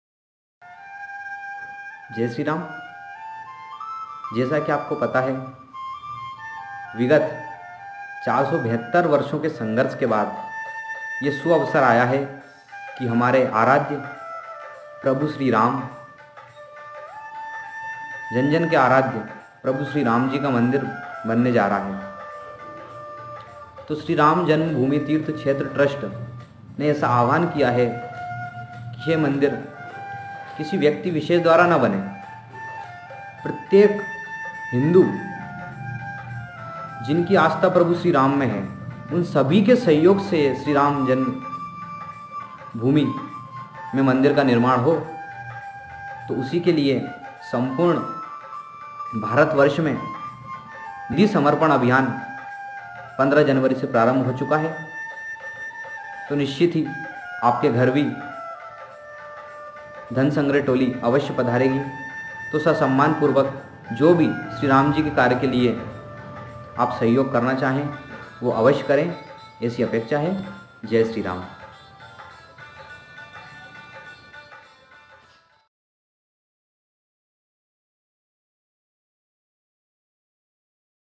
🙏🚩जय श्री राम 🚩🙏श्री राम मंदिर समर्पण निधि अभियान के लिए चाचौड़ा क्षेत्र के नव युवकों ने किया आवाहन
,🙏🚩जय श्री राम🚩🙏 श्री राम मंदिर समर्पण दीदी अभियान समर्पण निधि अभियान के लिए चाचौड़ा क्षेत्र की नव युवकों द्वारा हवन किया गया क्षेत्र की जनता से जय श्री राम champavati Nagri champavati Nagri YouTube channel चंपावती नगरी चंपावती नगरी यूट्यूब चैनल